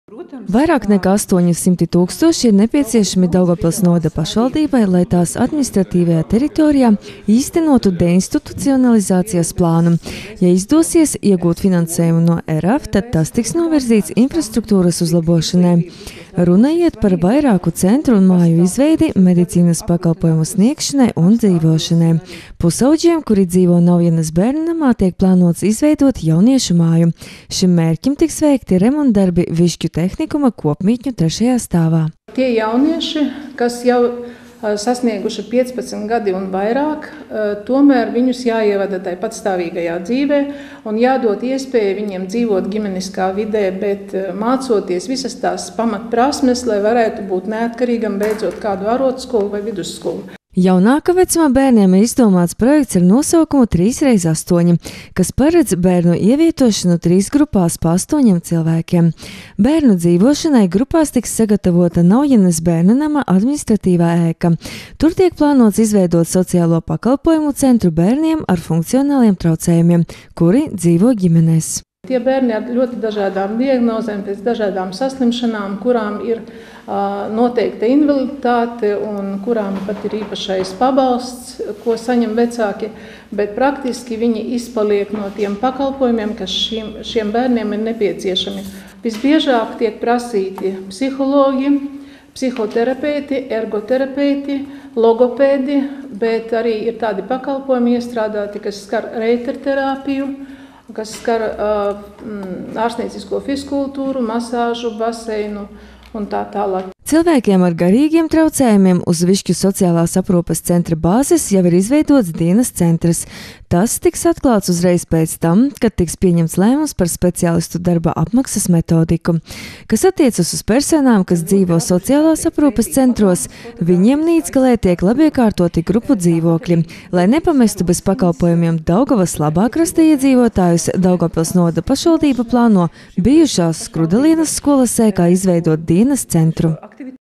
20.februārī aizvadīta ārkārtas sēde Daugavpils novada domē
20.februārī Daugavpils novada domē tika sasaukta ārkārtas sēde, kuras laikā tika pieņemti lēmumi par vairāku projektu iesniegšanu. Tie ir vērsti uz pārobežu sadarbību un infrastruktūras uzlabošanu.